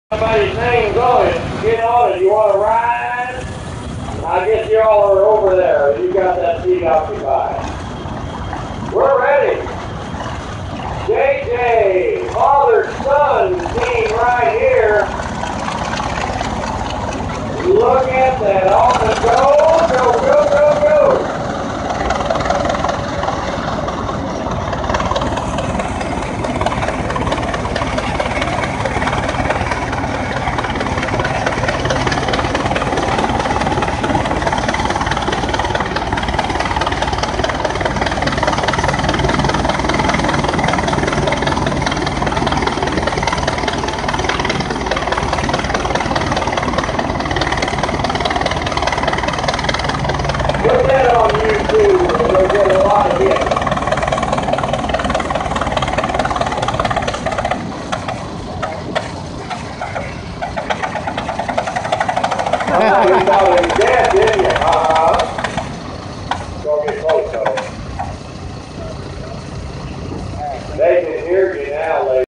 Auman Taylor tractor at old fashioned tractor pull
This is an old Auman Taylor gas tractor at the Missouri River Valley Old Threshers Association fall show held in Boonville, Missouri pulling an old fashioned walk-on sled. Video taken Saturday 9-7-13.